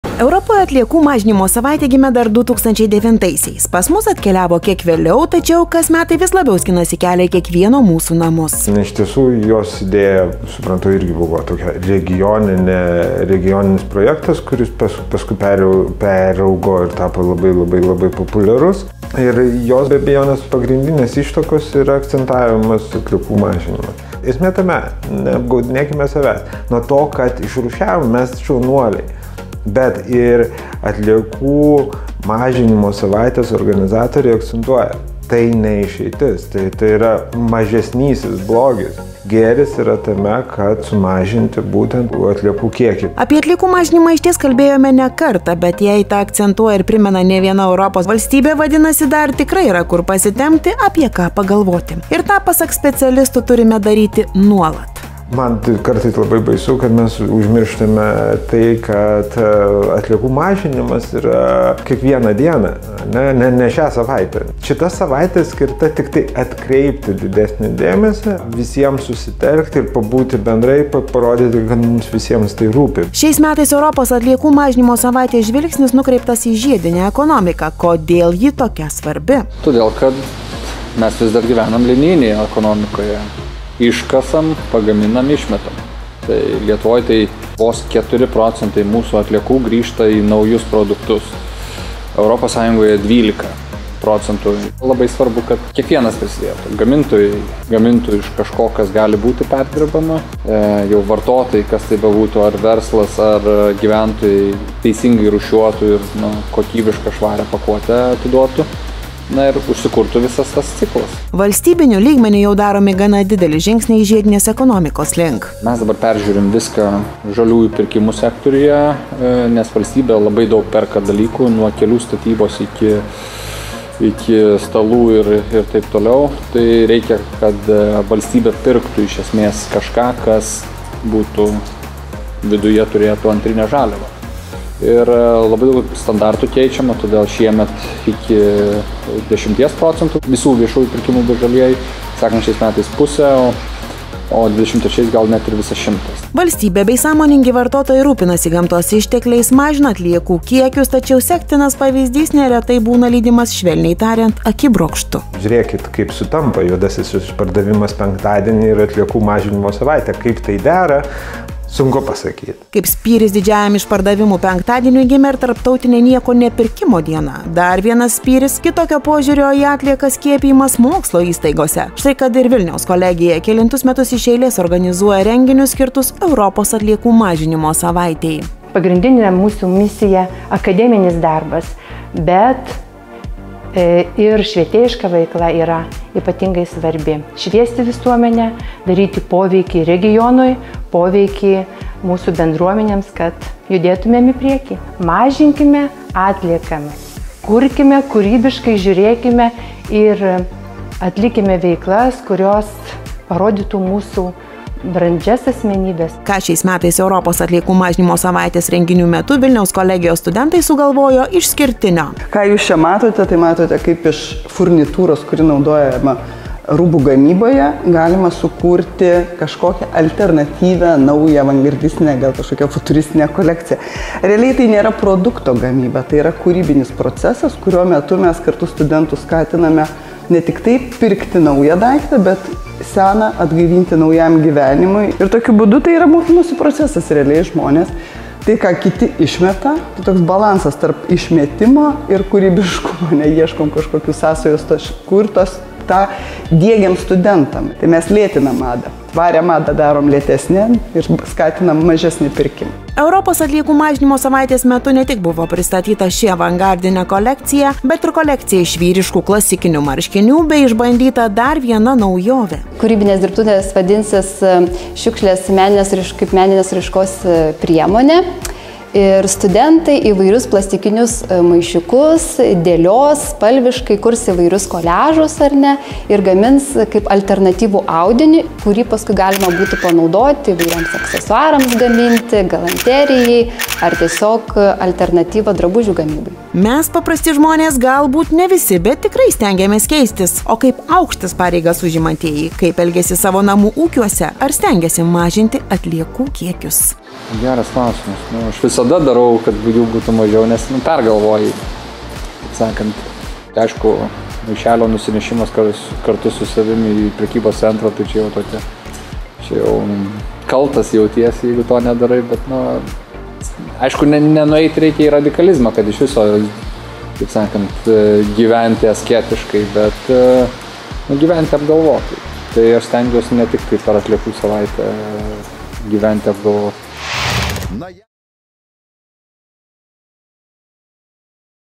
Europos atliekų mažinimo savaitė // Mintys ir renginiai
Šiais metais Europos atliekų mažinimo savaitės žvilgsnis nukreiptas į žiedinę ekonomiką. Kodėl ji tokia svarbi?

Mintimis dalinasi „Vilniaus apskrities atliekų tvarkymo centro“ direktorius Tomas Vaitkevičius, Aplinkos ministras Simonas Gentvilas, Vilniaus kolegijos Menų ir kūrybinių technologijų fakulteto dekanė Birutė Žygaitienė, Vilniaus kolegijos Mados dizaino katedros vedėja Silvija Grušienė ir Erika Gudienė, Vilniaus kolegijos Mados dizaino katedros lektorė.